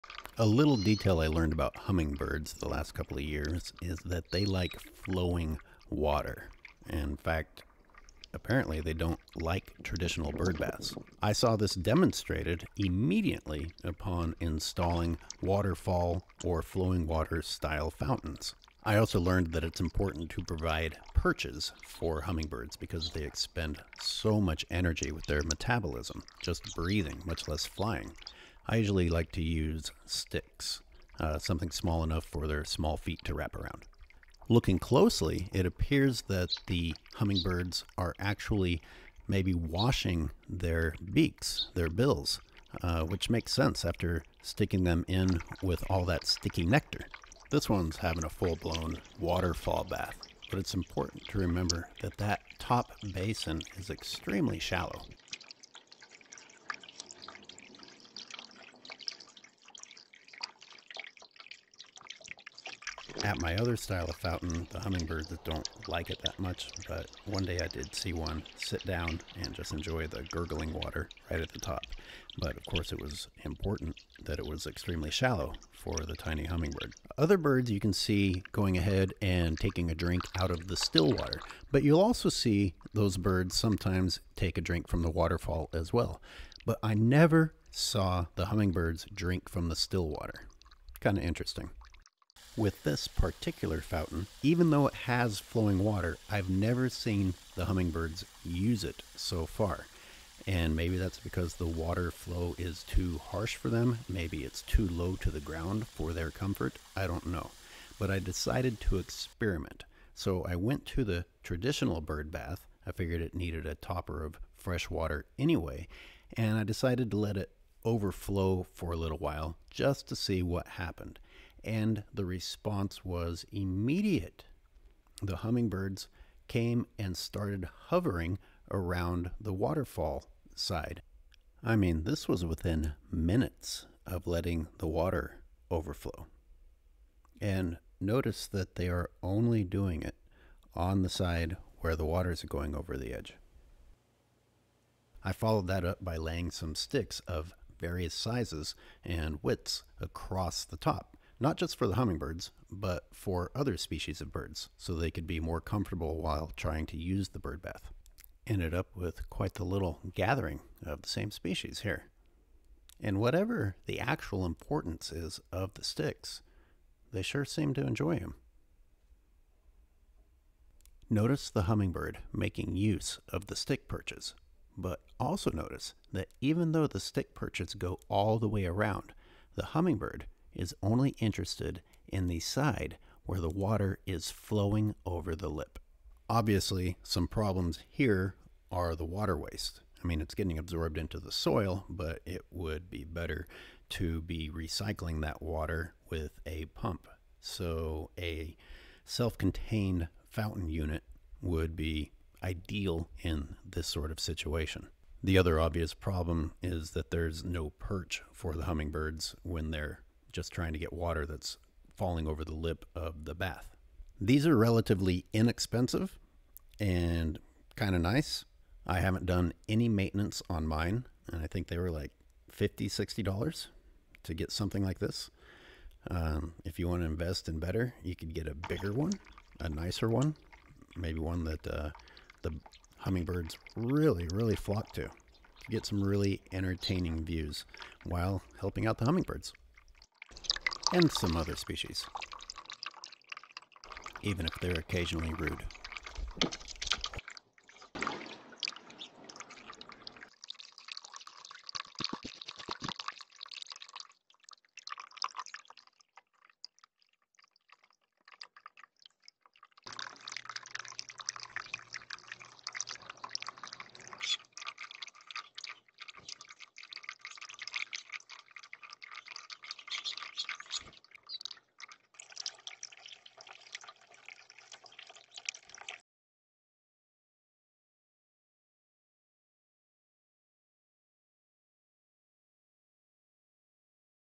Hummingbirds, Perches, and Flowing Water
What's the importance of flowing water to hummingbirds (and likely other species)?  And what about perches at birth baths/ bird fountains.
When I started reading how to attract hummingbirds a few years ago I learned that traditional bird baths are not their cup of tea.  Time and time again I have seen hummingbirds respond immediately to the provision of flowing water, such as a waterfall.  One reason is for the sake of washing the sticky nectar off.   But hey, other species drink from the waterfall as well...and why not?  Wouldn't we all prefer flowing water over still, stagnant water?